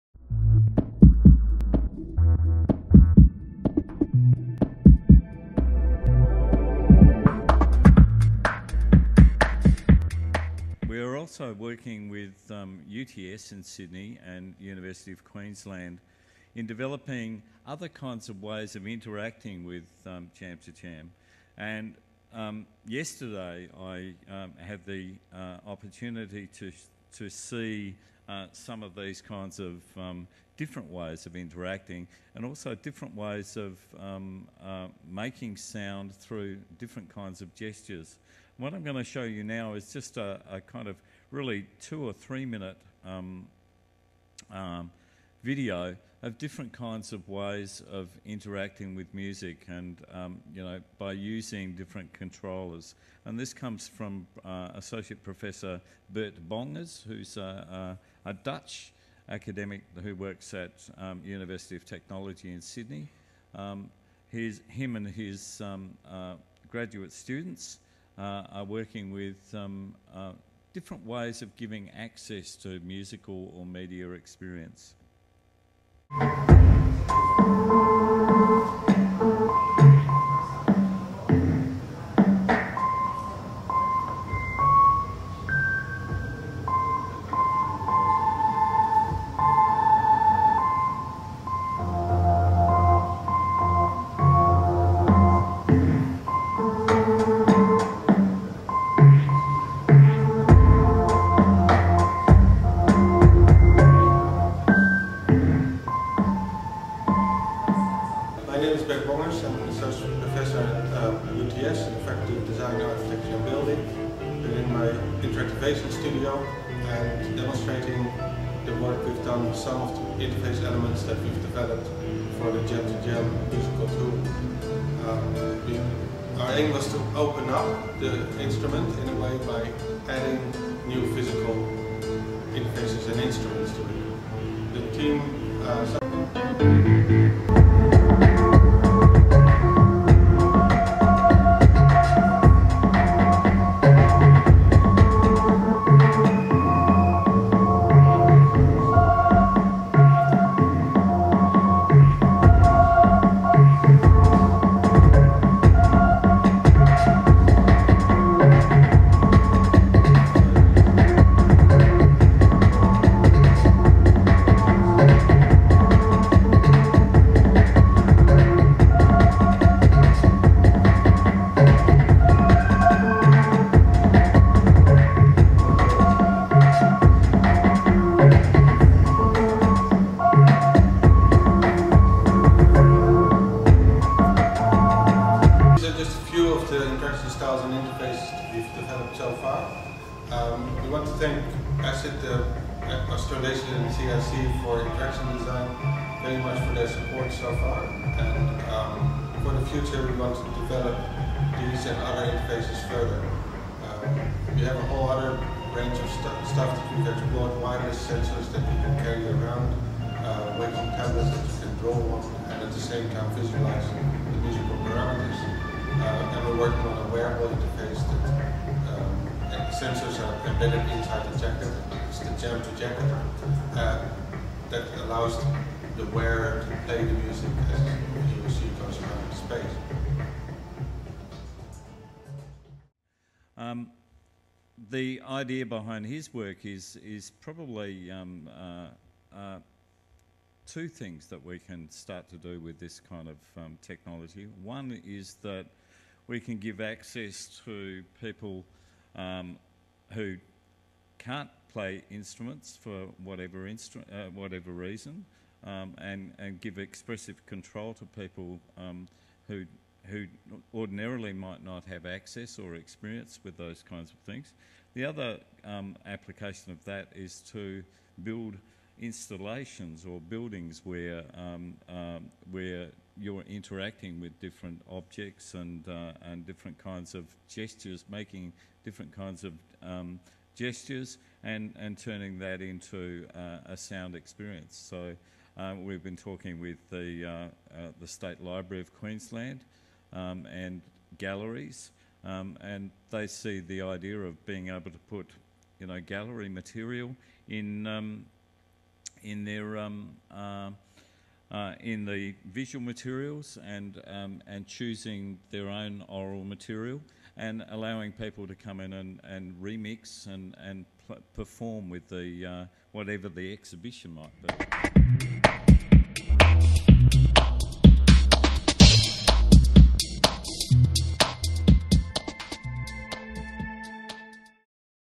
Bert bongers.m4v
Dr Steve Dillon Project leader forACID's  Network Jamming project introduces work by Associate Professor Bert Bongers from The University of Technology Sydney involving sensors and triggers for generative music/media performance using jam2jam. The presentation was part of the Jamskolan09 event that brought together researchers from Sweden the USA and Australia to develop applications and define a research agenda for generative media performance in schools and communities.